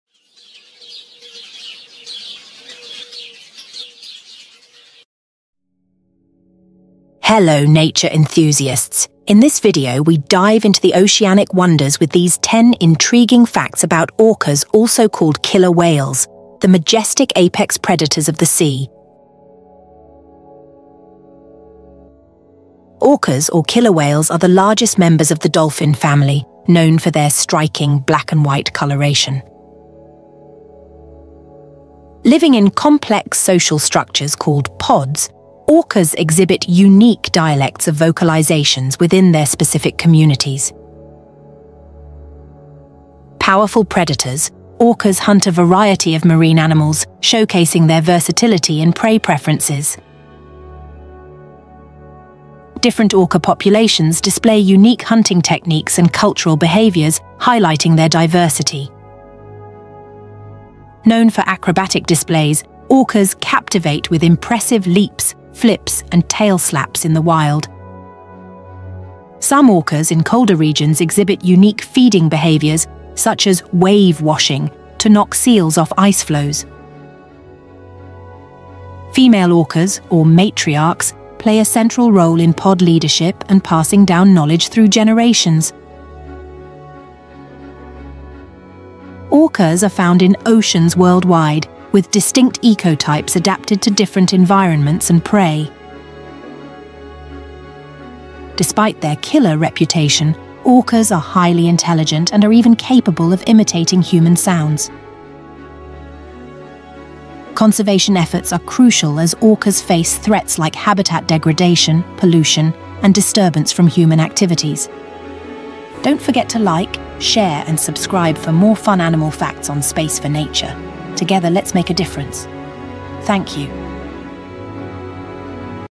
Orcas or Killer Whale | Animal Facts Series | Episode 23
Dive into the oceanic wonders with these 10 intriguing facts about Orcas, the majestic apex predators of the sea! From their complex social structures to remarkable hunting techniques, join us on a captivating journey beneath the waves.